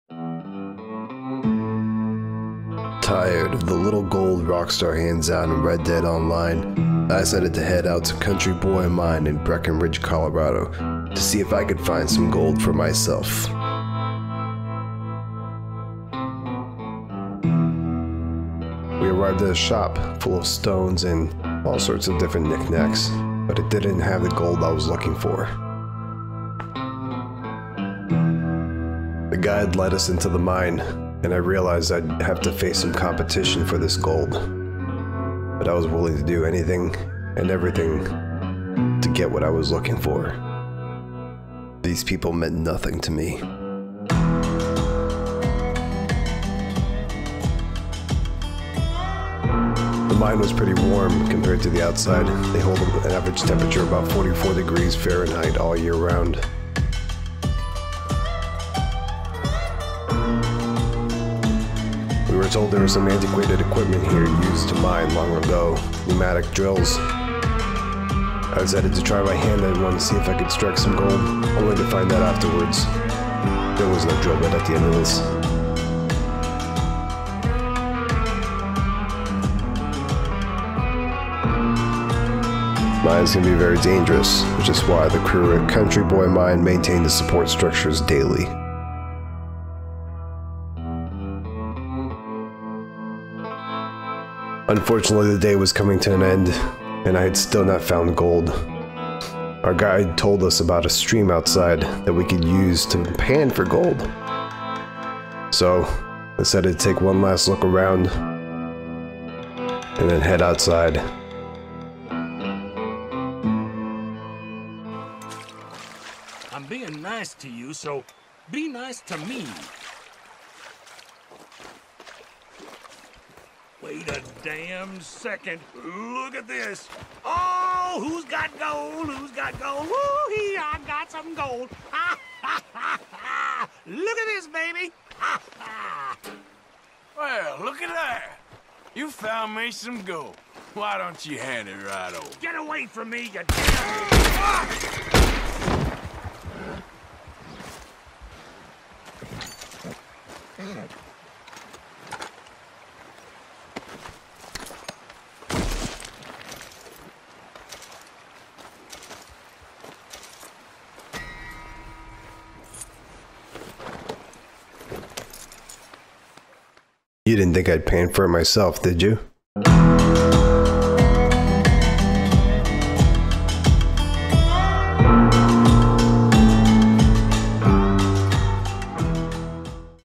Red Dead Online - Gold Mining IRL...
I take to Country Boy Mine in Breckenridge, Colorado to find gold, because lets be real, grinding for gold in Red Dead Online is the worst!